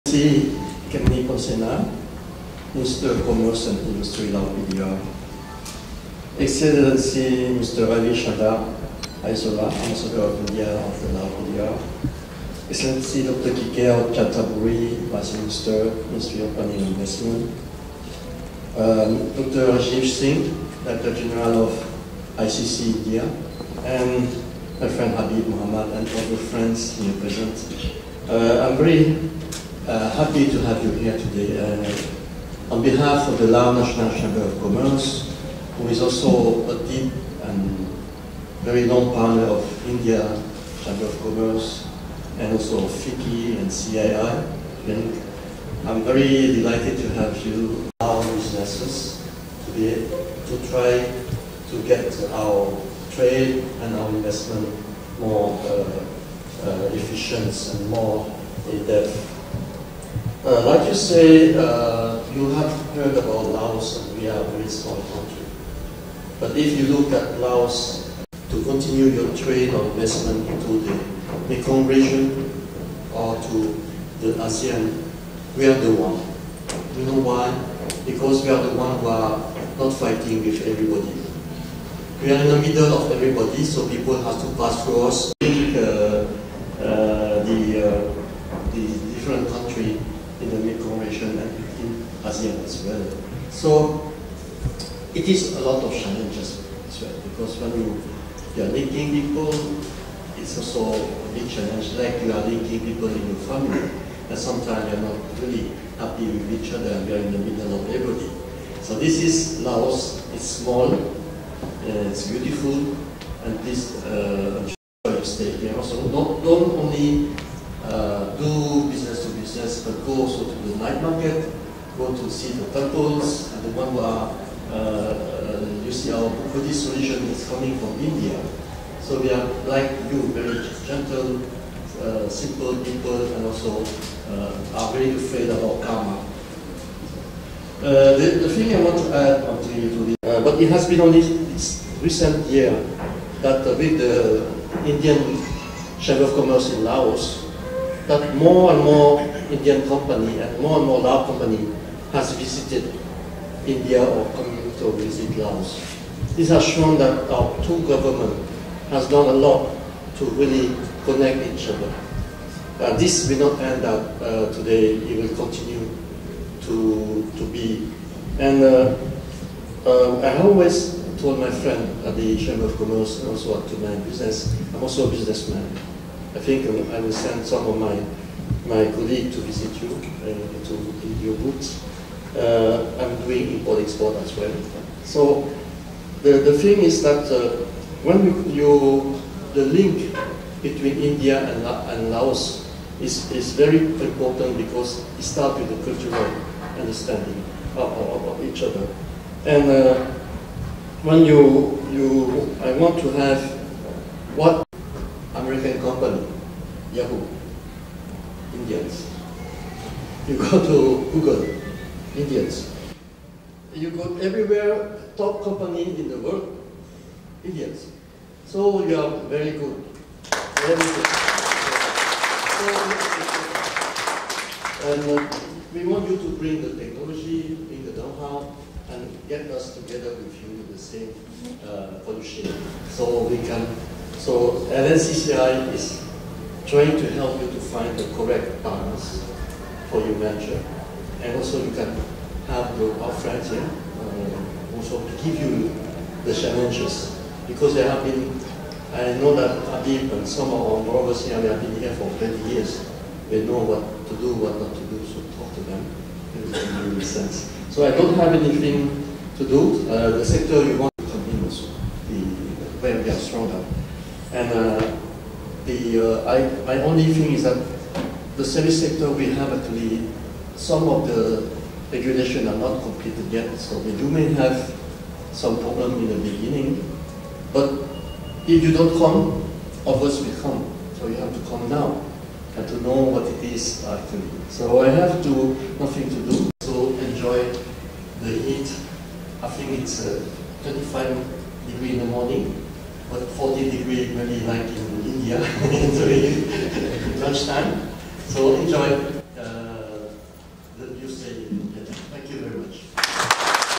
Mr. Oudet Suvannavong, Vice-President of LNCCI Laos at India Investrade 2016, Vientiane, Lao PDR
India Investrade 2016 in Lao PDR”
Exhibition and B2B Meetings
21st - 23rd March 2016 at Vientiane Center, Vientiane, Lao PDR

Inaugurated by

H.E. Madam Khemmani Pholsena, Minister, Ministry of Industry and Commerce, Lao PDR

H.E. Dr. Kikeo Chanthabury, Vice Minister, Ministry of Planning & Investment, Lao PDR

H.E. Mr Ravi Shankar Aisola, Ambassador, Embassy of Republic of India, Lao PDR

Mr. Oudet Suvannavong, Vice-President, Lao National Chamber of Commerce & Industry

Dr. Rajeev Singh, Director General, Indian Chamber of Commerce, Kolkata, India

Mr Habib Mohammed Chowdhury, Founder Chairman & Managing Director, HSMM Group of Companies